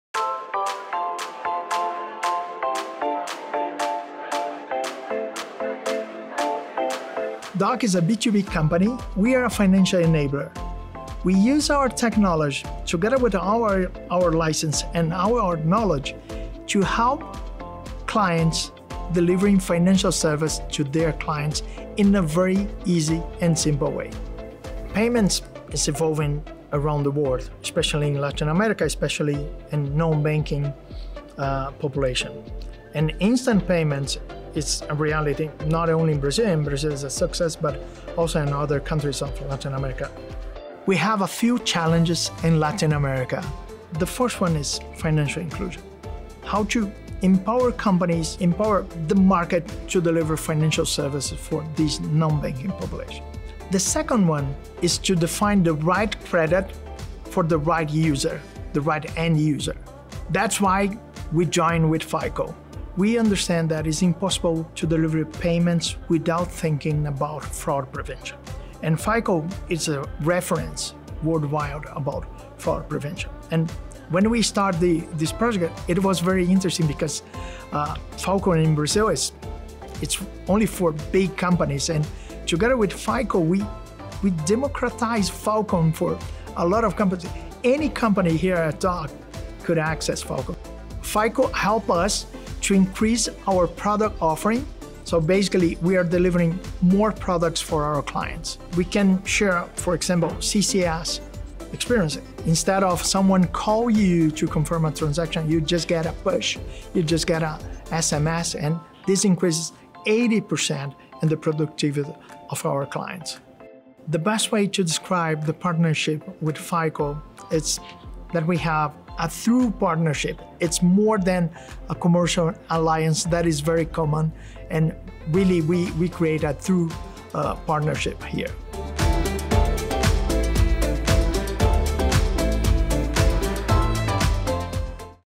Dock Stops Over USD$50m a Month in Fraud Using FICO Technology
Dock, a global technological platform for financial services, is using FICO fraud prevention technologies to stop more than USD$50 million a month in fraudulent transactions for its clients. The innovative pay-as-you-go solution allows Dock’s clients, including banks, fintechs, and retailers, to intercept fraudulent transactions and protect their operations, while also improving the user experience.